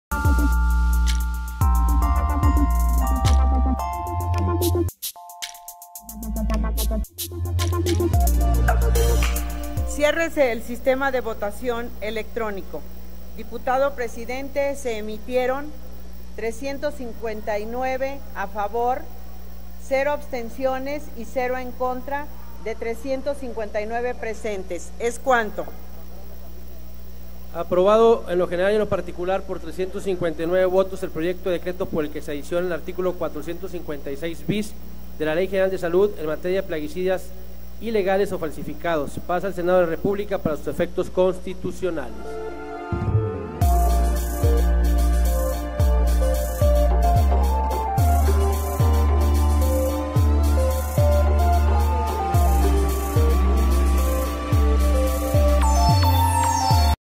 Aprobación Adicionar el art. 456 Bis a la Ley General de Salud
En votación nominal se aprueba con 359 votos a favor, 0 en contra y 0 abstenciones, el dictamen por el que adiciona el artículo 456 Bis a la Ley General de Salud, en materia de plaguicidas ilegales o falsificados.
Aprobado en lo general y en lo particular el dictamen por el que adiciona el artículo 456 Bis a la Ley General de Salud, en materia de plaguicidas ilegales o falsificados.
Se remite a la Cámara de Senadores para sus efectos constitucionales.